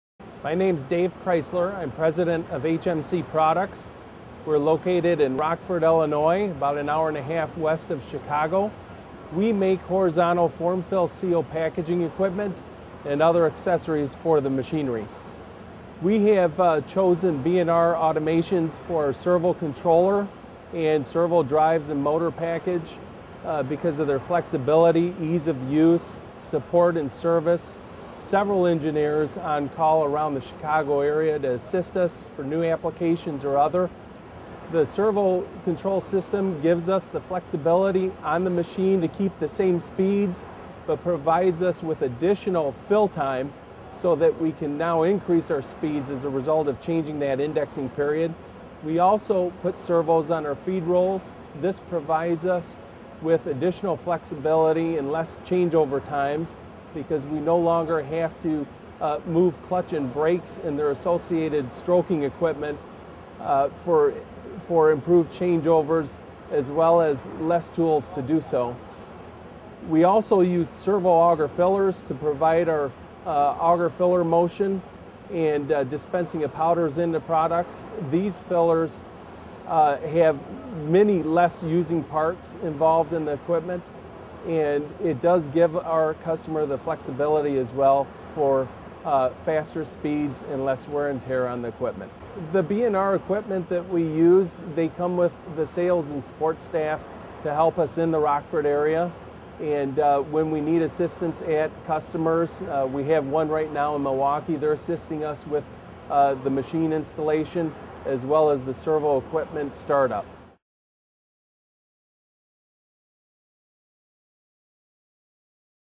Automated by B&R - HMC Products @ Pack Expo 2010
Dave Kreissler explains the advantages of using B&R.